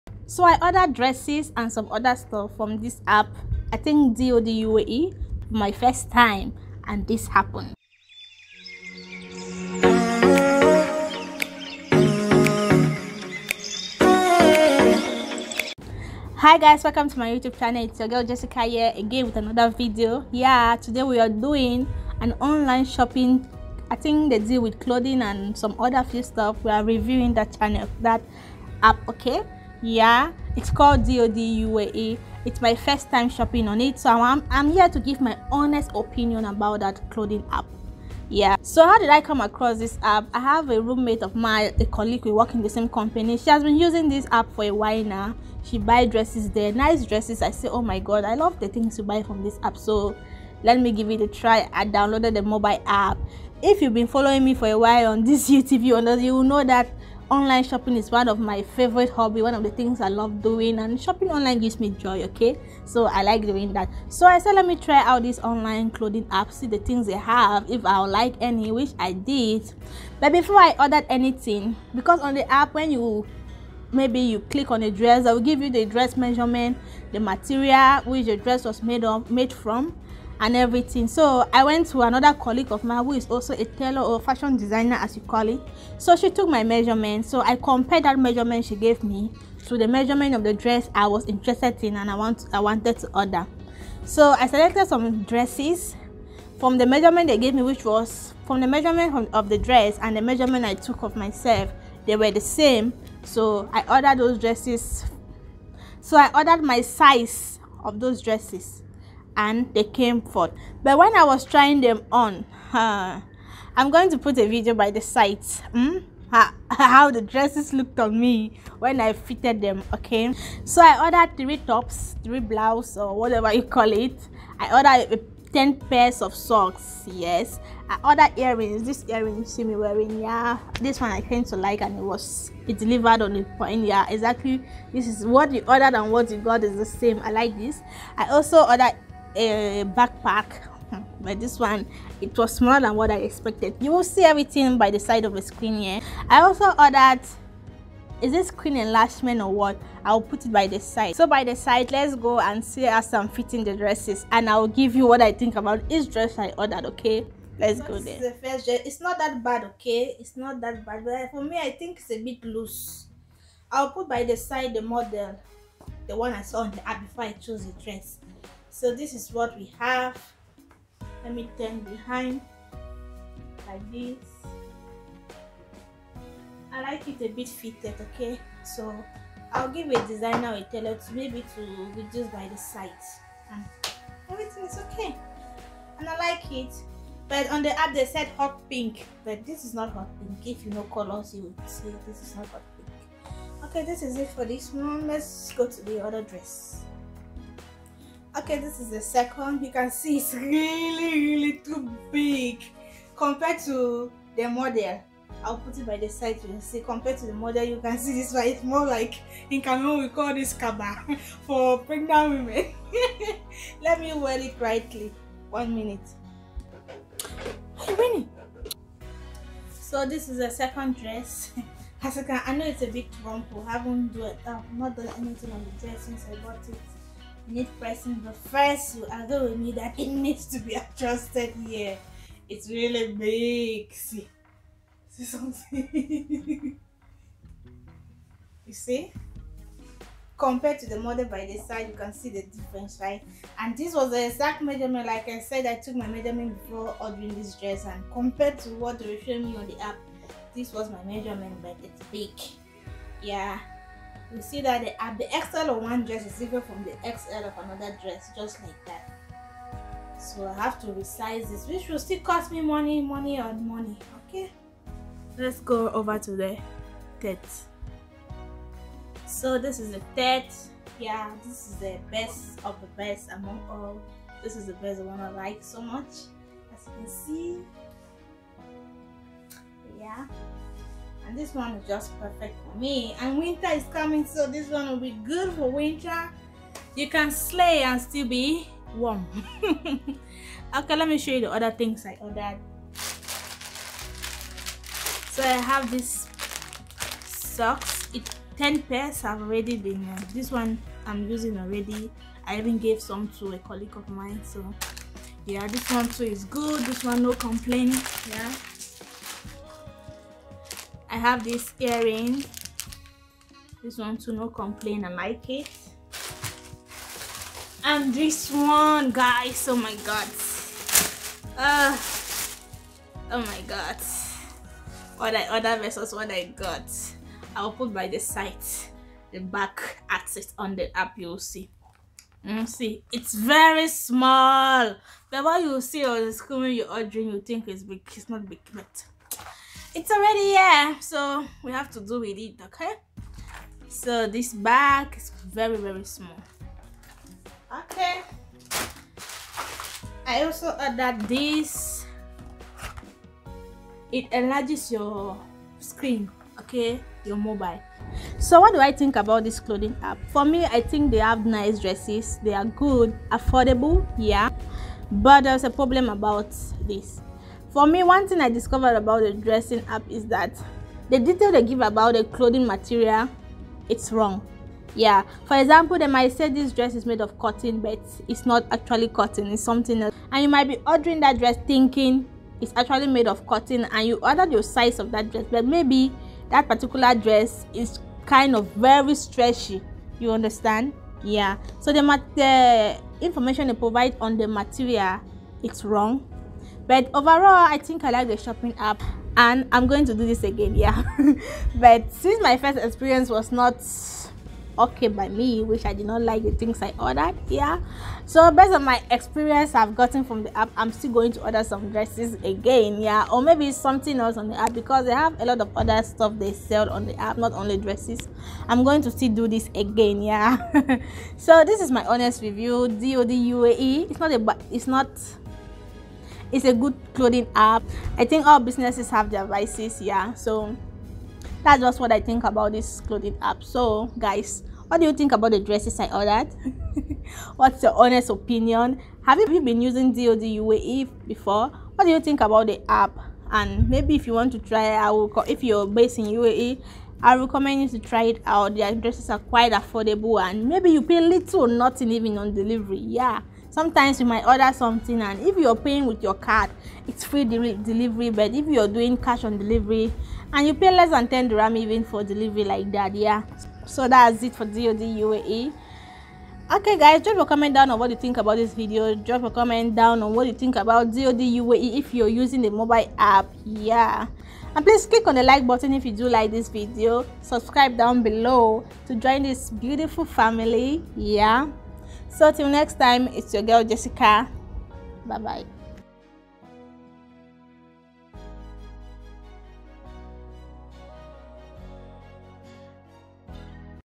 DODuae Online Shopping Try-On Haul/ Review ||What I ordered Vs What Was Delivered...It's Jesseca
DODuae Online Shopping Try-On Haul/Review. My honest opinion about Shopping Online with DODuae.
See what I ordered Vs What was delivered ‼